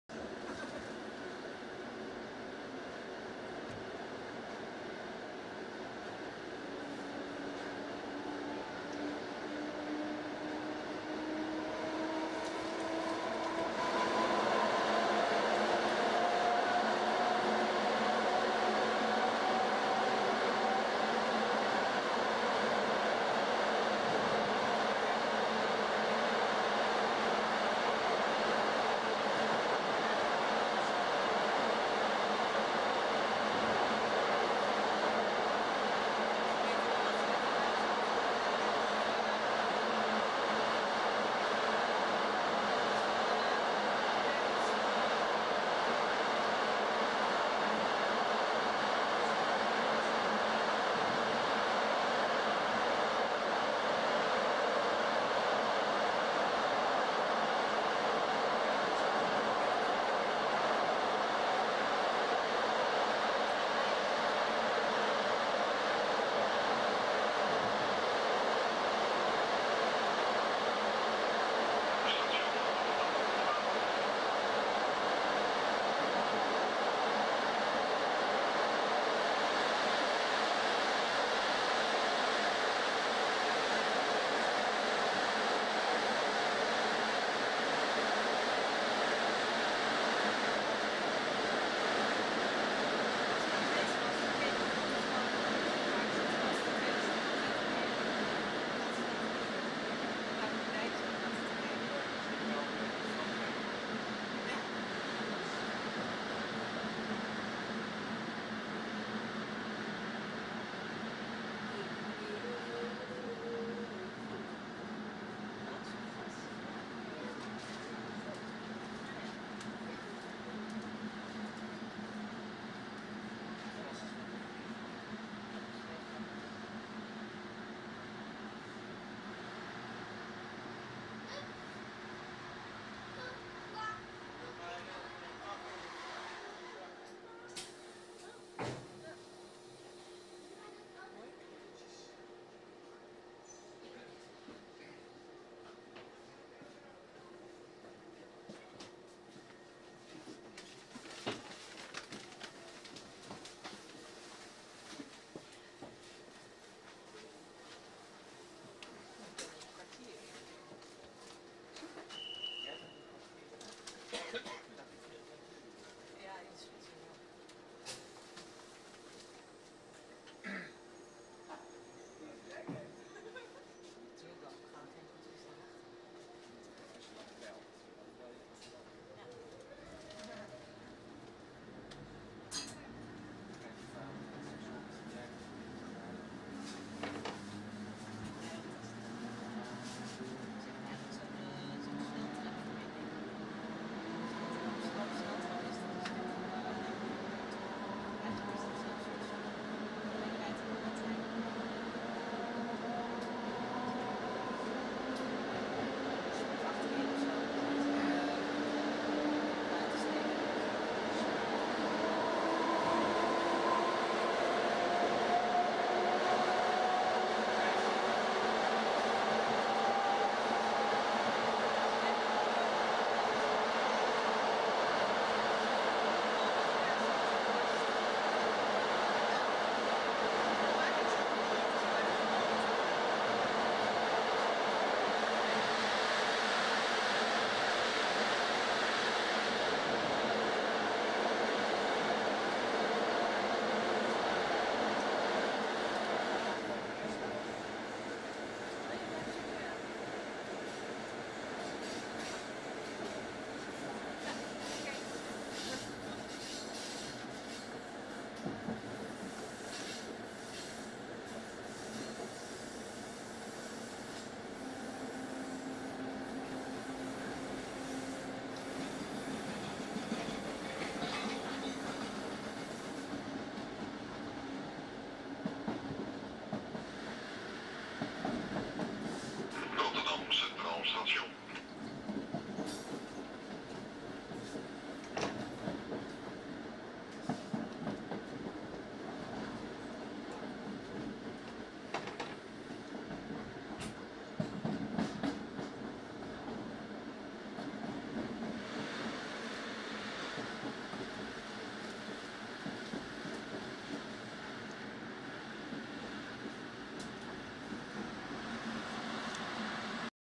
Het nieuwe uitzicht
Van Rotterdam-Zuid naar Centraal door de tunnel: 4-sporig! Wat een architectuur!